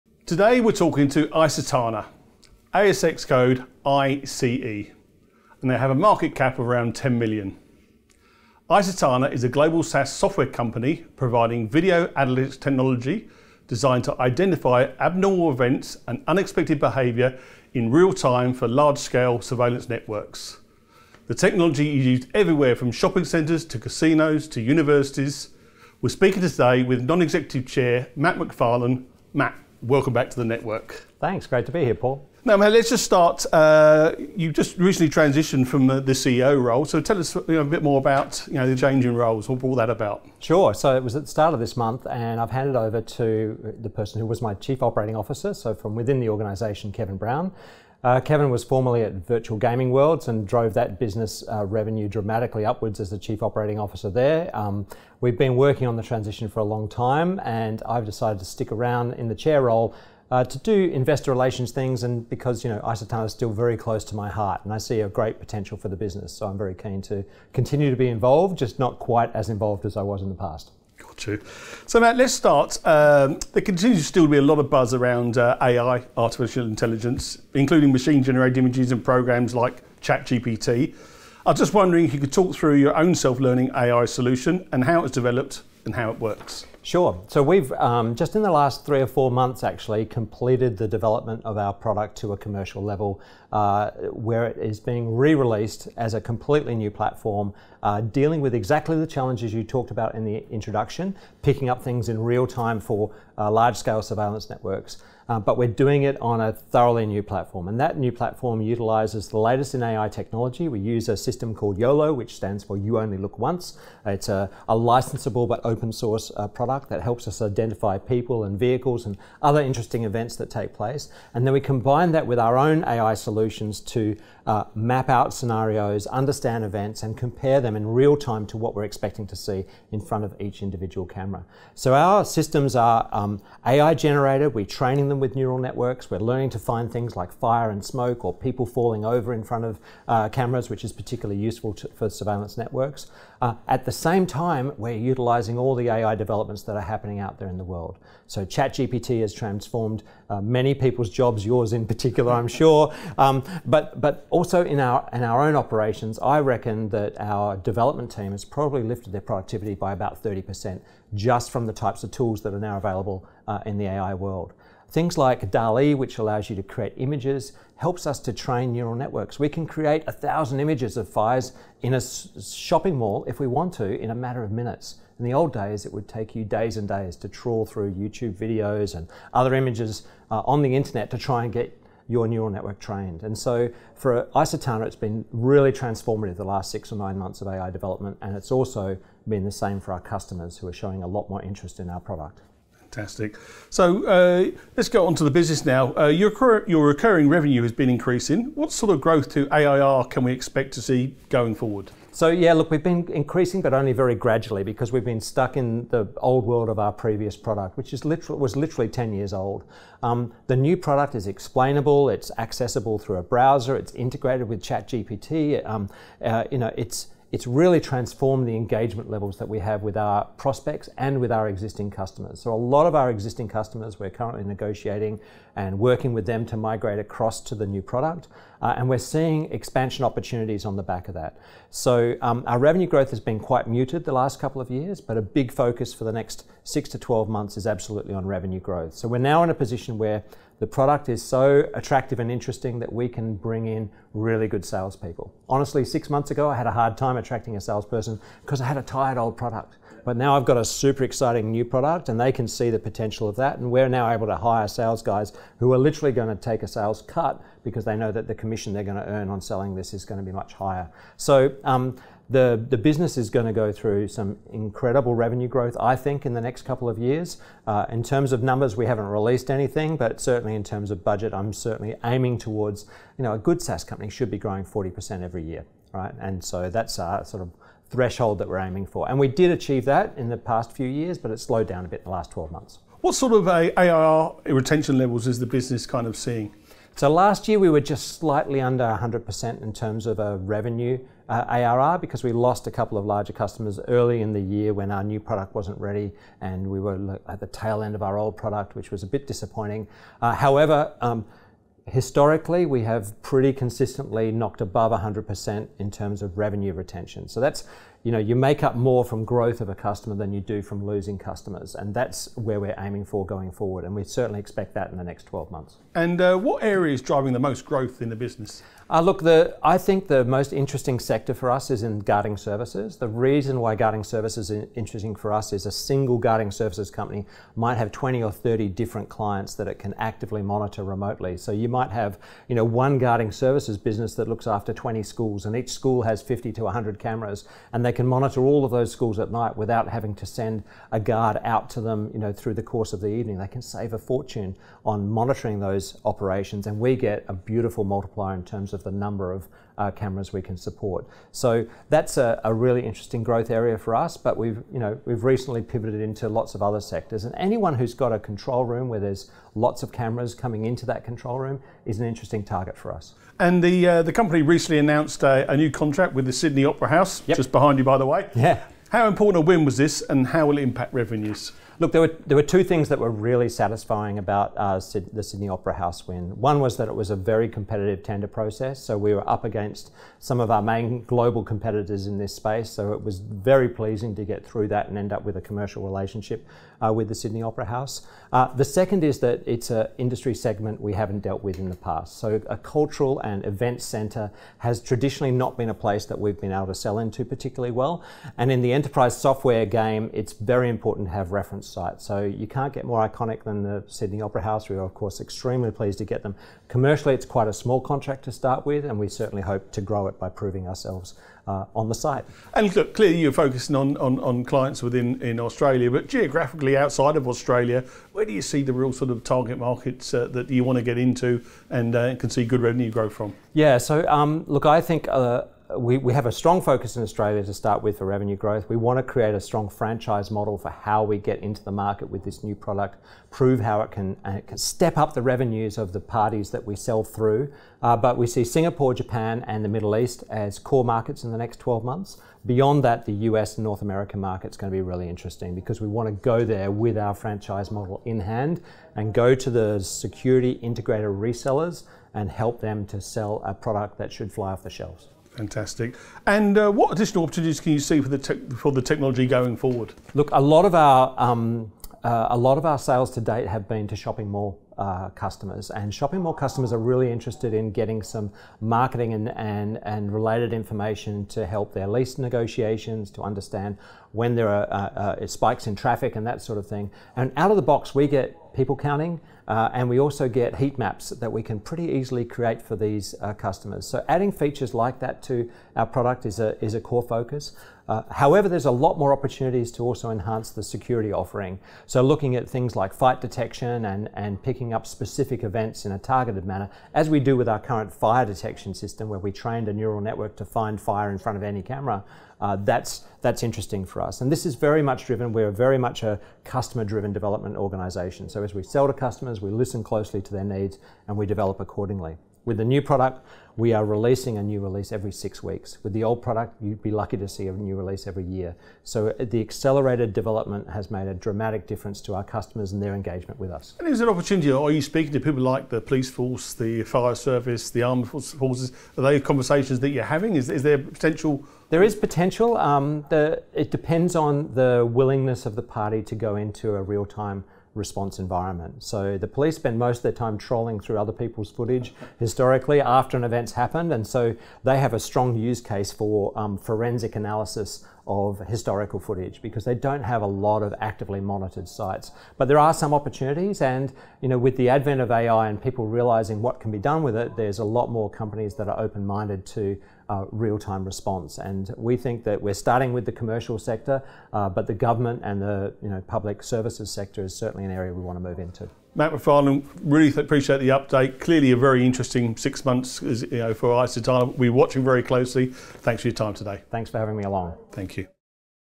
icetana (ASX:ICE) secures Sydney Opera House contract
icetana Limited (ASX:ICE) Non-Executive Chair Matt Macfarlane discusses his transition from the CEO role, the company's artificial intelligence solution, annual recurring revenue growth, and the recent contract secured with the Sydney Opera House.